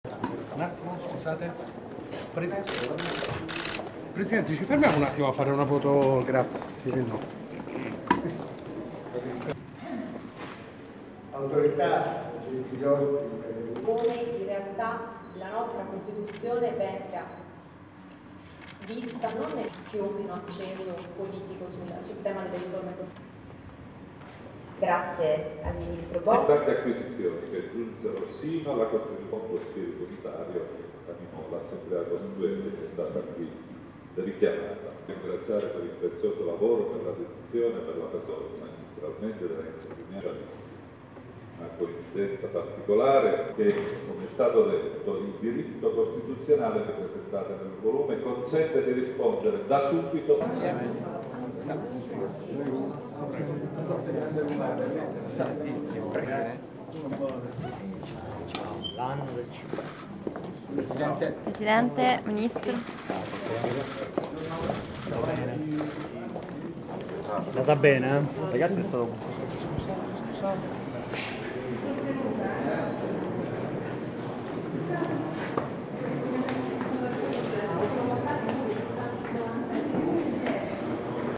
MARIA ELENA BOSCHI E GRASSO INSIEME AL CONVEGNO SULLA COSTITUZIONE
(Agenzia VISTA) - Roma, 22 Settembre 2015 - Il Presidente del Senato, Pietro Grasso, e il Ministro delle Riforme Costituzionali Maria Elena Boschi, durante la presentazione a Palazzo Giustiniani del volume "Le costituzioni italiane. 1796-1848".